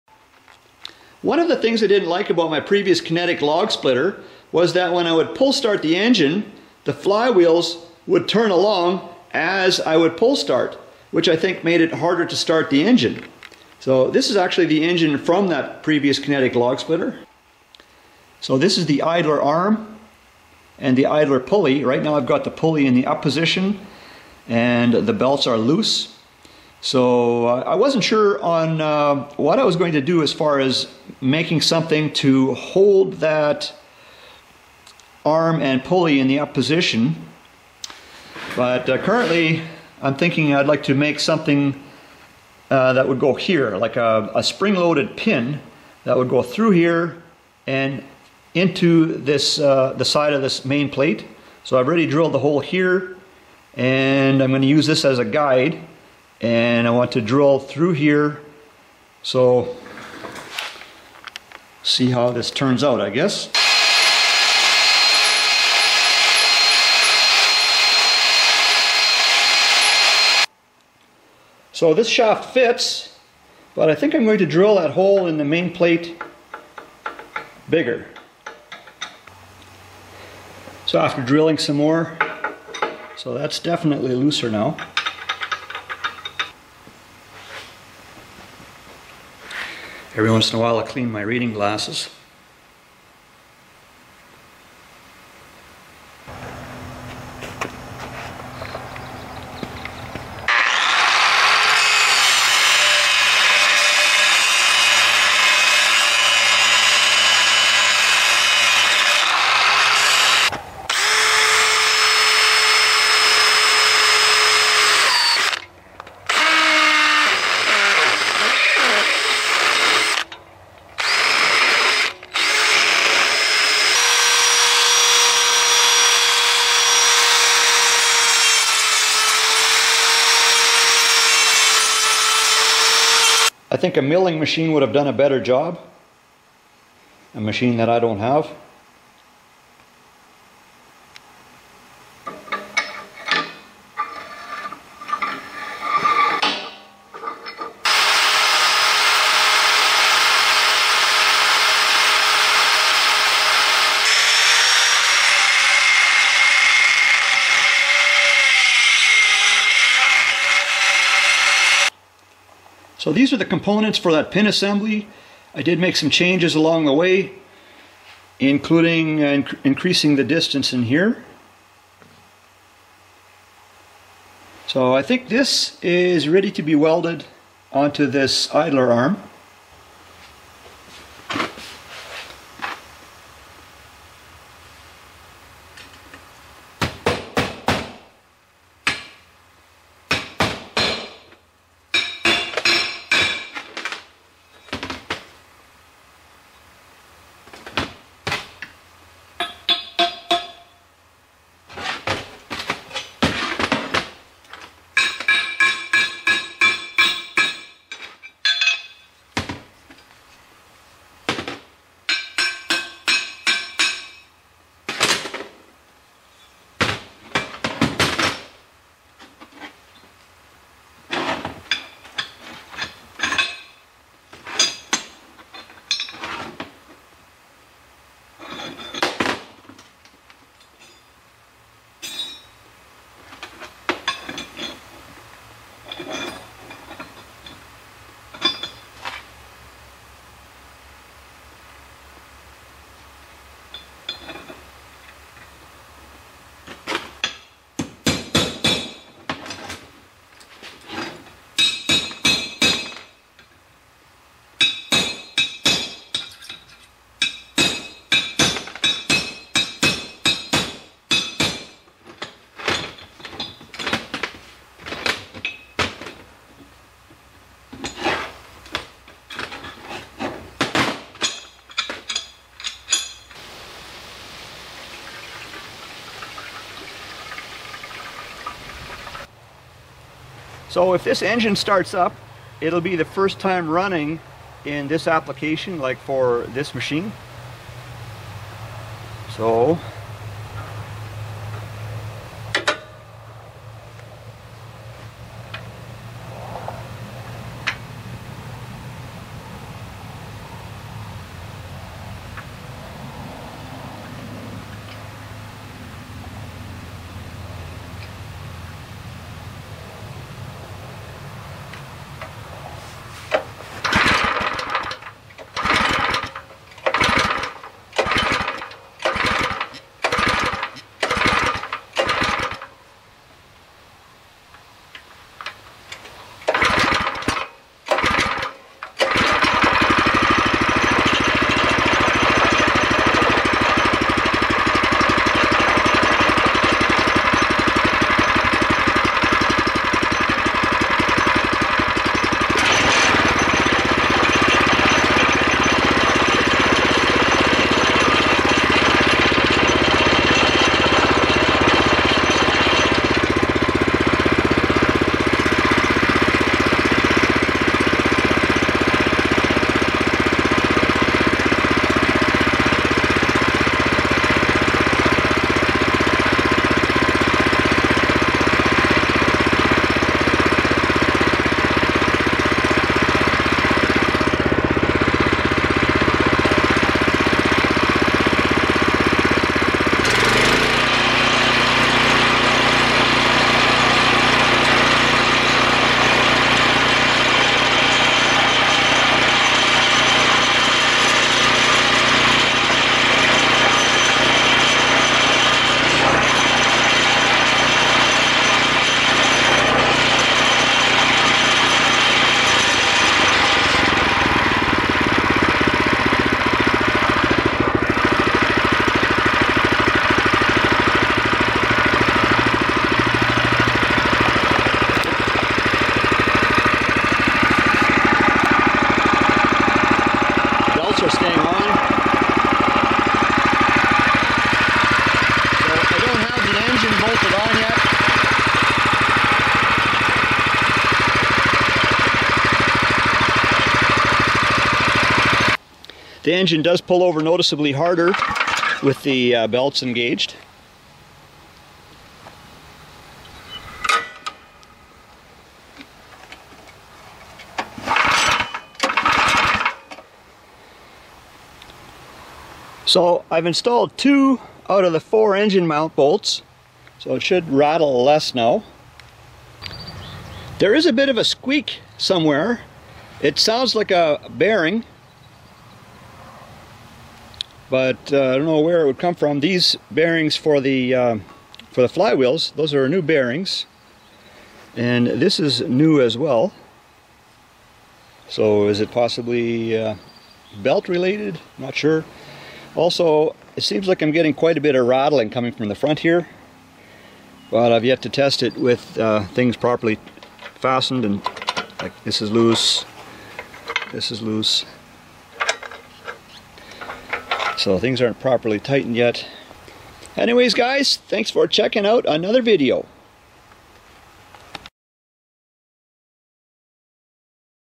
Kinetic Log Splitter Build Part 24
Idler tension and first start of incomplete machine